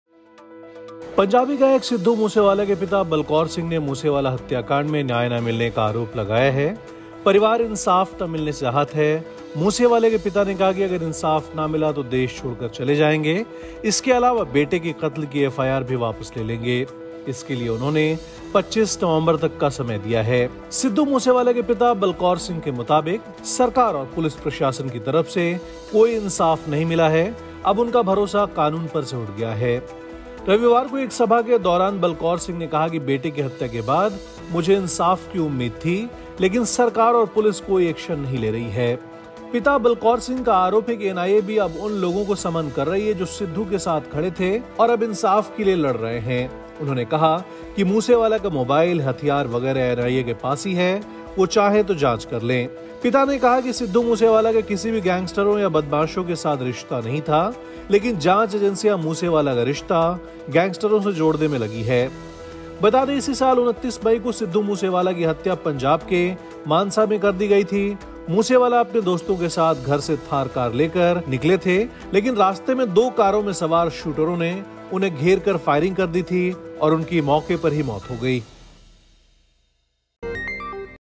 देश छोड़ देगा मूसेवाला का परिवार! पिता बलकौर सिंह बोले- पुलिस नहीं दे पाई न्याय
सिद्धू मूसेवाला के पिता बलकौर सिंह बेटे की मौत का इंसाफ न मिलने से आहत हैं. बलकौर सिंह ने पंजाब पुलिस को 25 नवंबर तक का अल्टीमेटम दिया है. उन्होंने कहा कि अगर एक महीने में उन्हें इंसाफ नहीं मिला तो वो परिवार के साथ देश छोड़ देंगे.